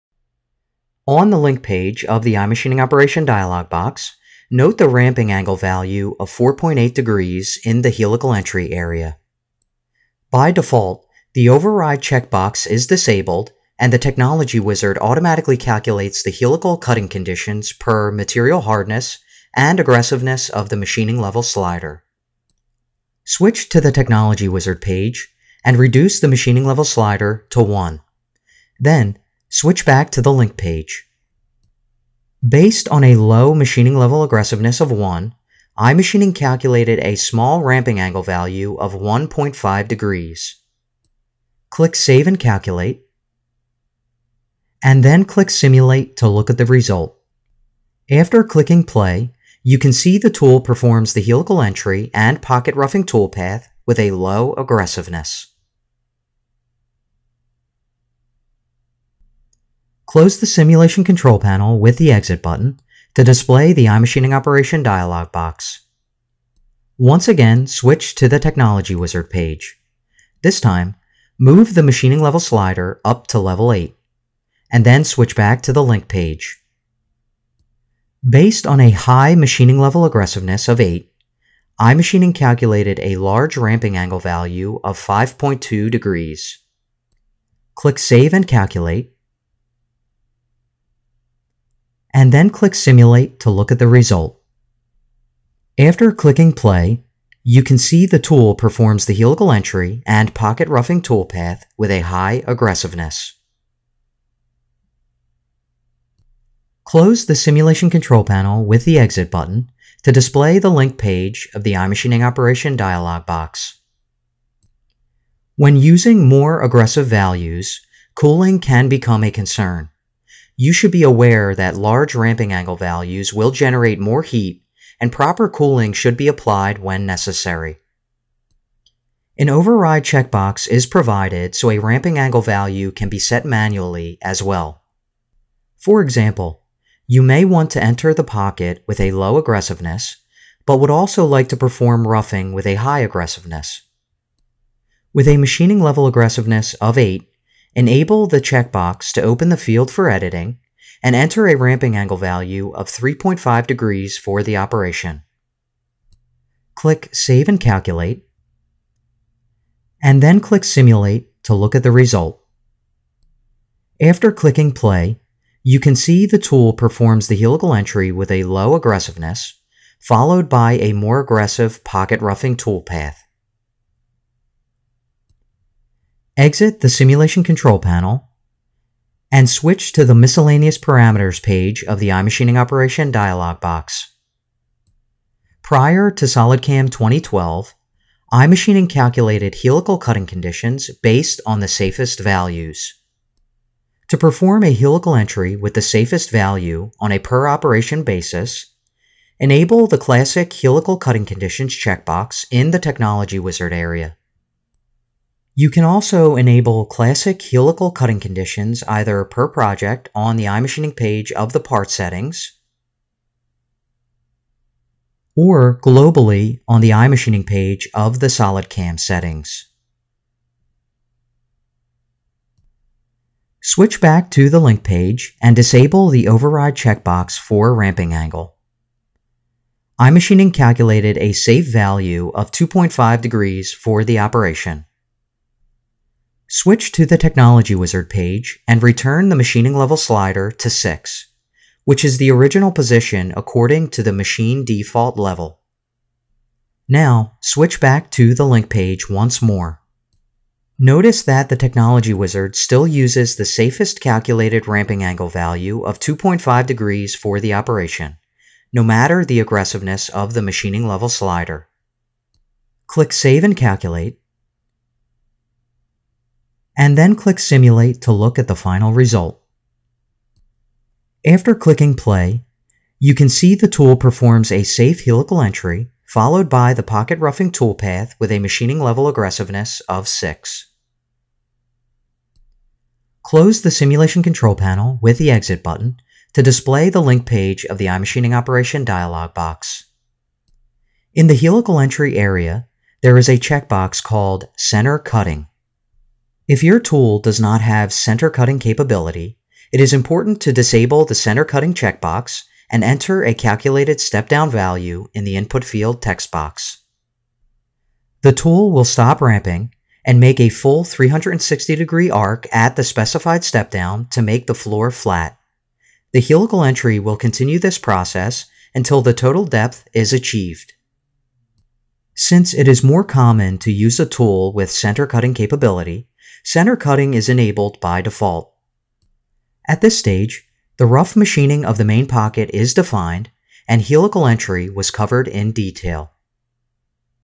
SolidCAM iMachining Training Course - Exer #5: Helical Entry
This video is Step 2.1 in Exercise #5 of the Interactive Training Course for iMachining. The iMachining Technology Wizard automatically calculates the helical cutting conditions per material hardness and aggressiveness of the Machining level slider. Helical Entry is covered in detail and is shown from one extreme (level 1) to the other (level 8).